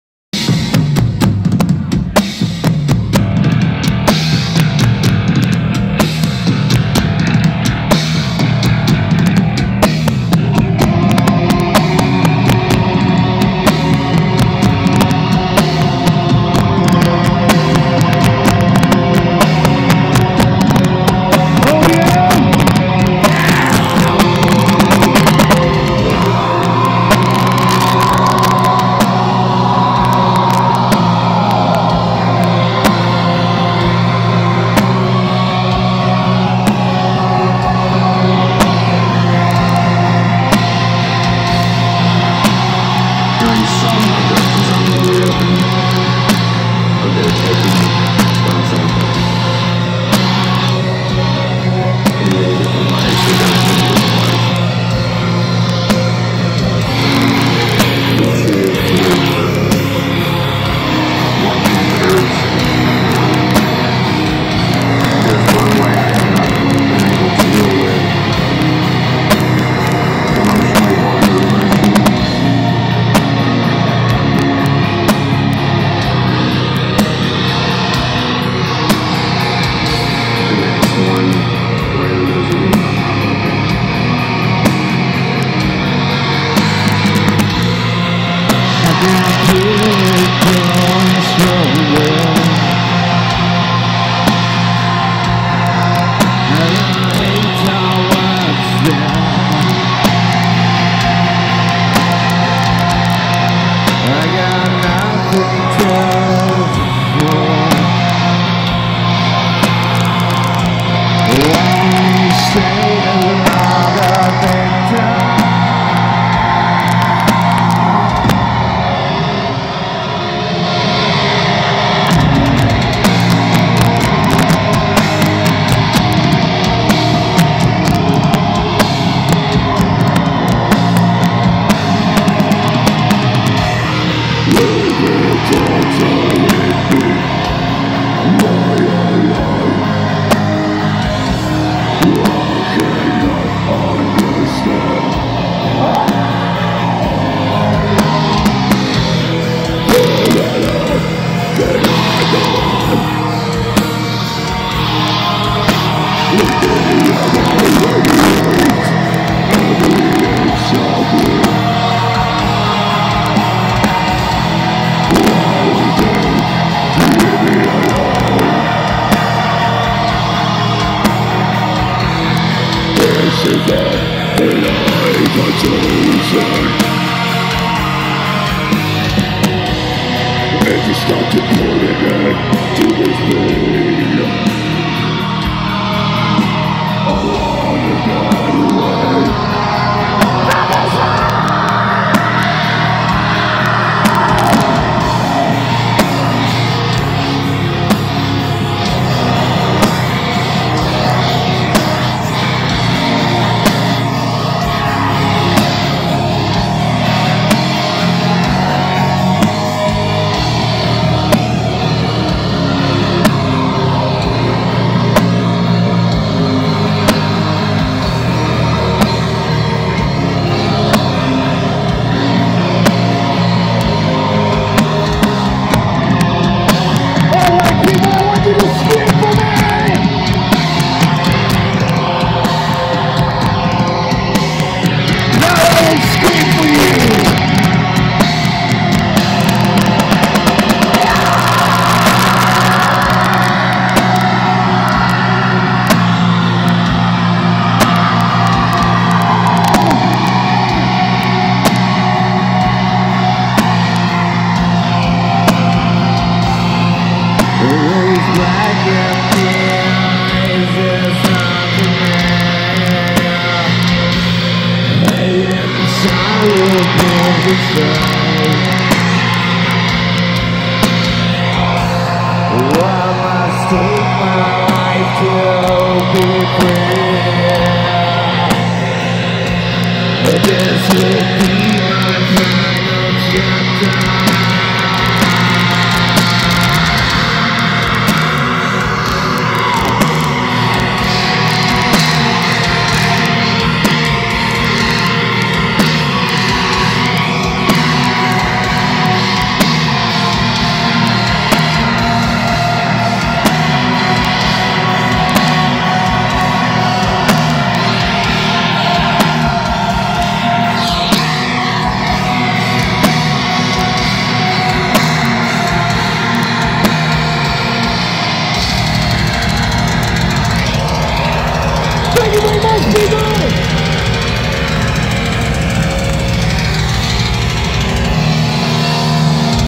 HYPOCRISY AT COMEANDTAKEITLIVE on March 23rd 42019?
I was toast and it was my first hypocrisy concert but got wasted but it was a badass time Haha